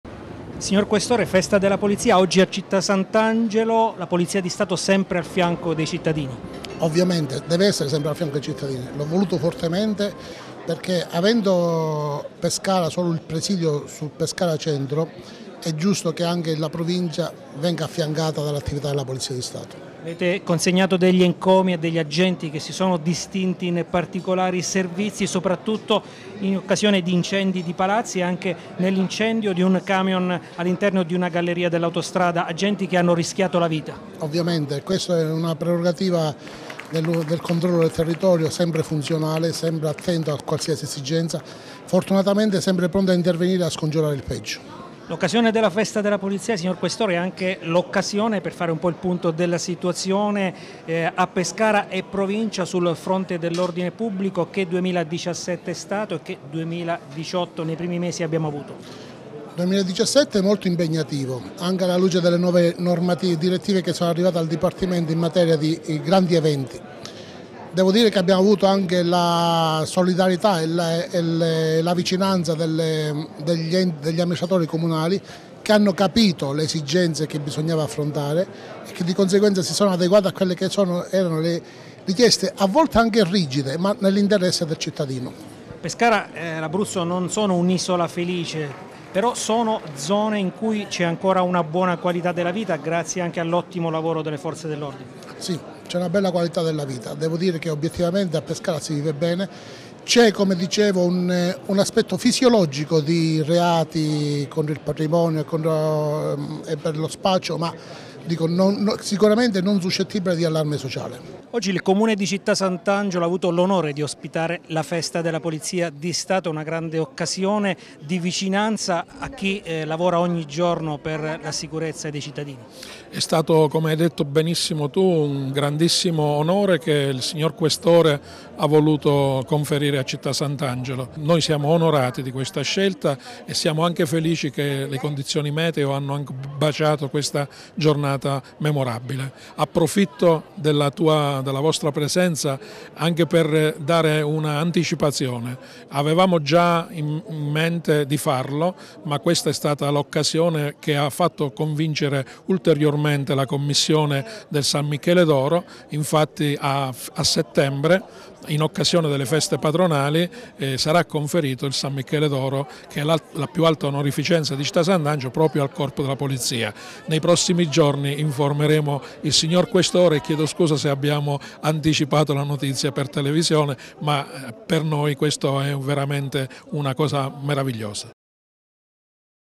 Festa della Polizia. Cerimonia a Città Sant'Angelo
La tradizionale cerimonia della Festa della Polizia, per il 166esimo anniversario del Corpo, quest'anno si è tenuta a Città Sant'Angelo. Premiati dal Questore di Pescara gli agenti che si sono distinti in particolari operazioni.